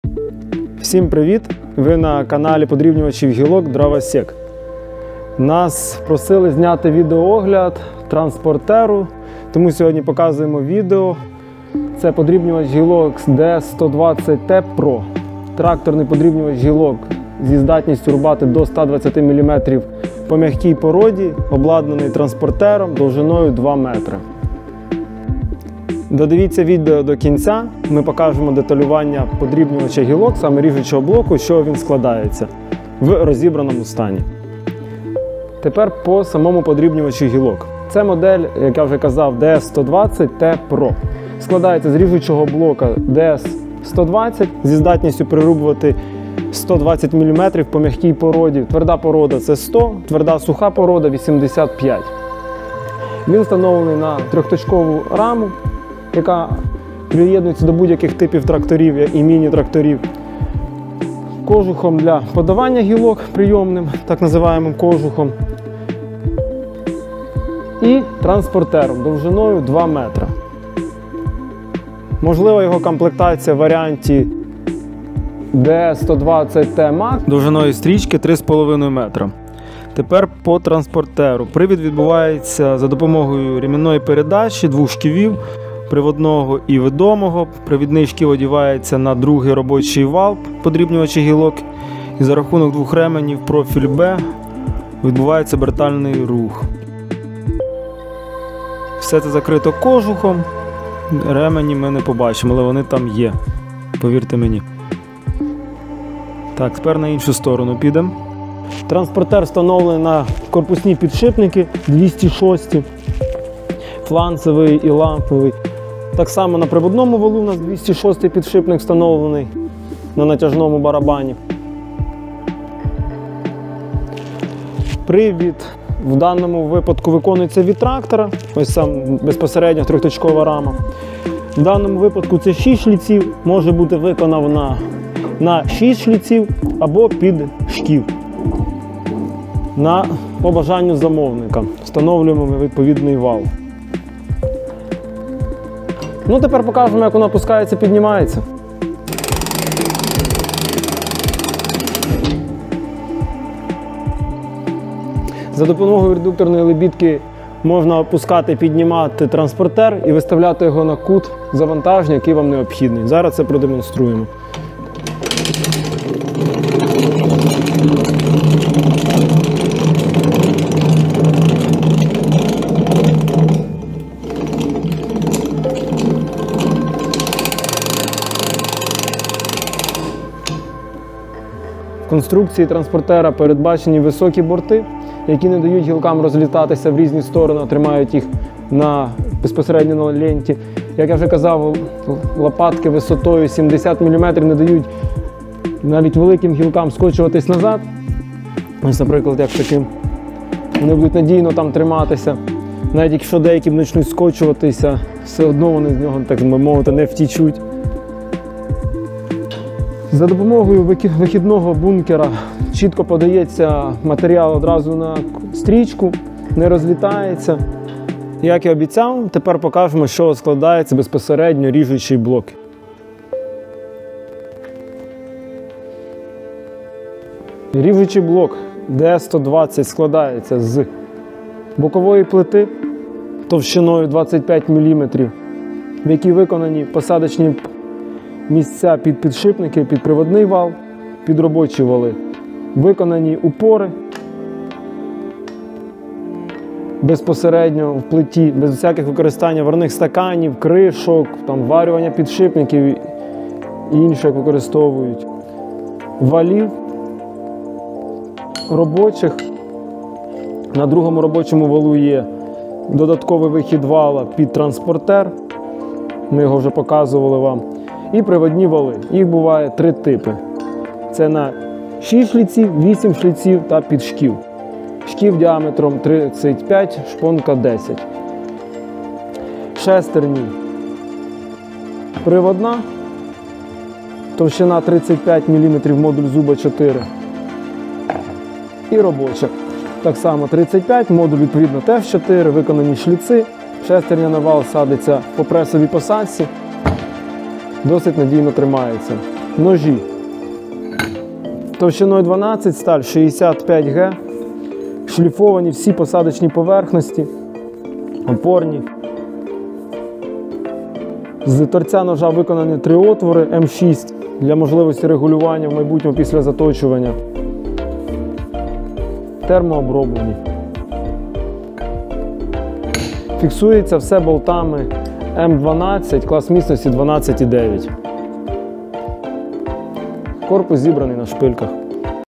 [ДЕТАЛЬНО] подрібнювач гілок З ТРАНСПОРТЕРОМ. Відео огляд ДРОВОСЄК ДС-120ТPRO.
Як влаштований подрібнювач гілок з транспортером виробництва Дровосєк? Детально відео огляд моделі ДС-120ТPRO з конвеєром довжиною 2 м. Гілкоподрібнювач до 120 мм з приводом від ВВП працює в тандемі з трактором потужністю 25 - 50 к. с. Більш детальний опис даної моделі на сайті

Купивши подрібнювач гілок з транспортером дровосєк ви отримаєте наступні переваги:
👌 прийнятна ціна;
👌 висока продуктивність;
👌 якість з гарантією перевірена часом;
👌 незалежність від ціни на газ в опалювальний сезон.